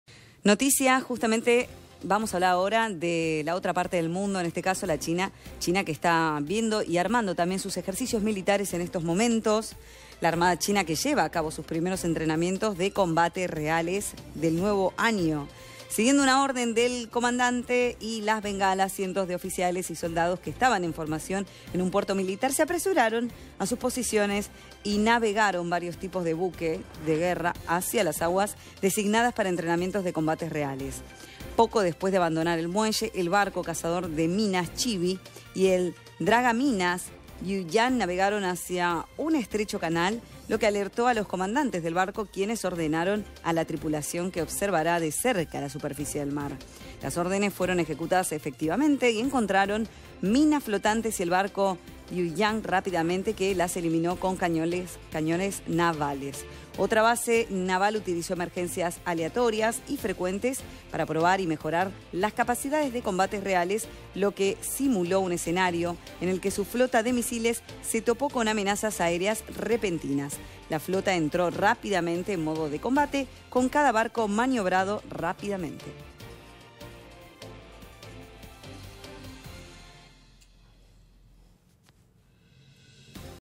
China realiza nuevos ejercicios militares
👉 El gigante asiático desarrollo entrenamientos de combate reales.